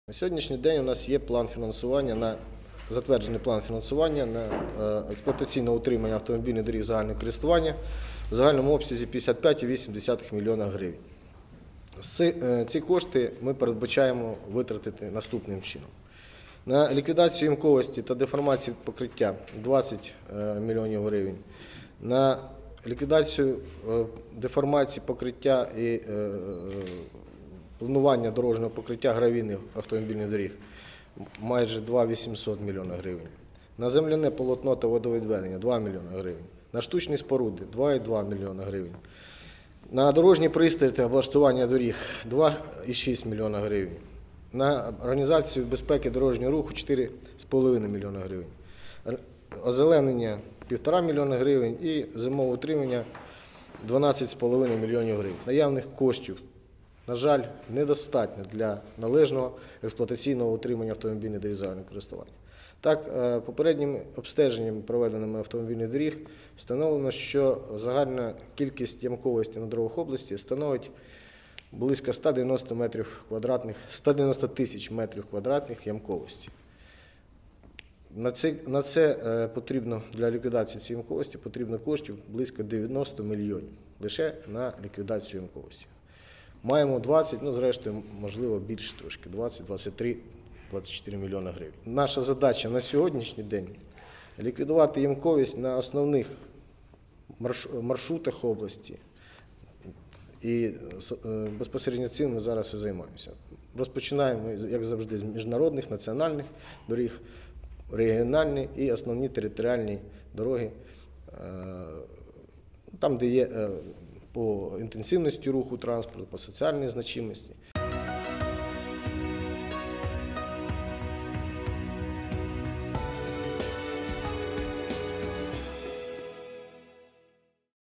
На ямковий ремонт буковинських доріг потрібно близько 90 мільйонів гривень,а є трохи більше 20-ти
Дорогам Чернівецької області цього року знову не пощастило, адже держава виділила на їхній ремонт всього 55, 8 млн. грн. Попри незначну суму , планується ліквідувати ямковість,  провести деформацію дорожнього полотна, а також провести ряд заходів по безпеці дорожнього руху.
Як розповів начальник Служби автомобільних доріг у Чернівецькій області Ігор Гах, нині Буковині потрібно 90 млн.грн. для ямкових робіт, які складають близько 190 тис. м.кв.
«Наша задача ліквідувати ямковість на основних маршрутах. Розпочинаємо як завжди з міжнародних, національних, регіональних і основних територіальних доріг », - сказав Ігор Гах.
Посадовець зазначив, що цього кварталу ремонт доріг розпочався дещо раніше, ніж зазвичай, а саме з березня, що дало змогу провести ремонтні роботи на 6 тис. м. кв.
Також Ігор Гах розповів, що держава планує виділити близько 175 млн. грн., аби провести капітальні  ремонти у різних напрямках Чернівецької області.